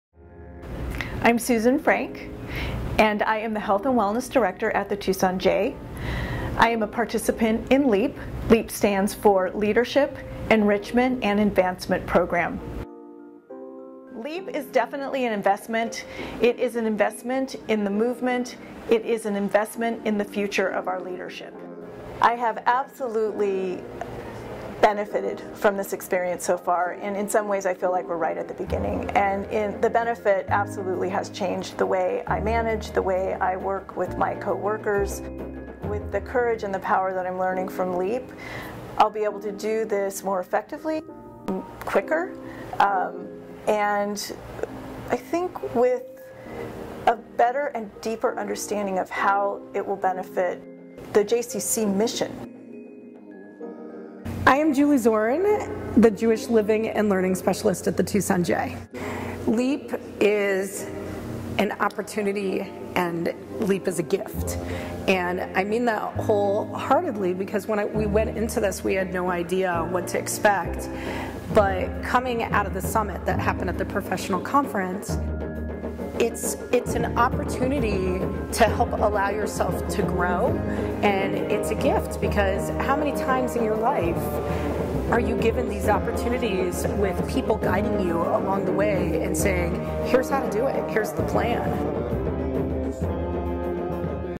Tucson JCC - JCCA LEAP Program
Julie and Susan talk about the LEAP Program of which they are participants.